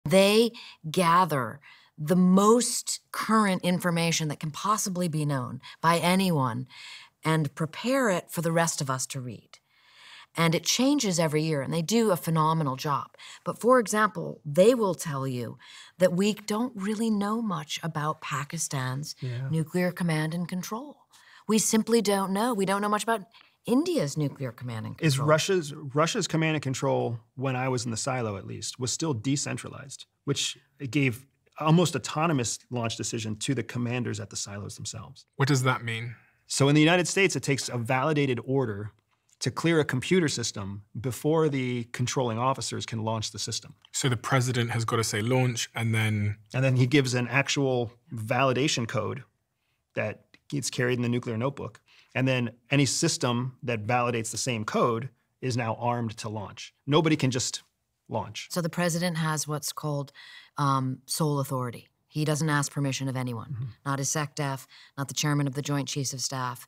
Inside Nuclear Launch Codes: President's Sole Authority Revealed!
Experts compile the latest nuclear intelligence, but critical gaps remain. Pakistan, India, even Russia—how secure are their launch protocols? Decentralized systems raise unsettling questions about autonomous decisions.